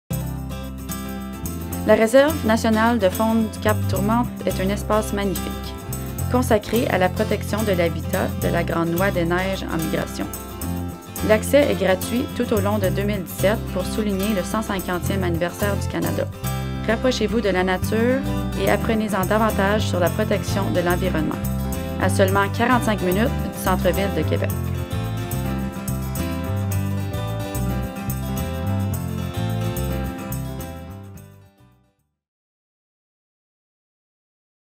Cap-Tourmente RNF est gratuite en 2017!
L’accès est gratuit toute l’année 2017 à la réserve nationale de faune du Cap-Tourmente . Célébrez Canada 150 avec nous. Cap-Tourmente, un joyau dédié à protéger l’habitat de la Grande Oie des neiges en migration. Nouez des liens avec la nature et apprenez à protéger notre environnement. À 45 minutes du centre-ville de Québec.

Transcription:

La Réserve nationale de faune du Cap-Tourmente est un espace magnifique consacré à la protection de l’habitat de la Grande Oie des neiges en migration.

L’accès est gratuit tout au long de 2017 pour souligner le 150e anniversaire du Canada.

Rapprochez-vous de la nature et apprenez-en davantage sur la protection de l’environnement.

À seulement 45 minutes du centre-ville de Québec.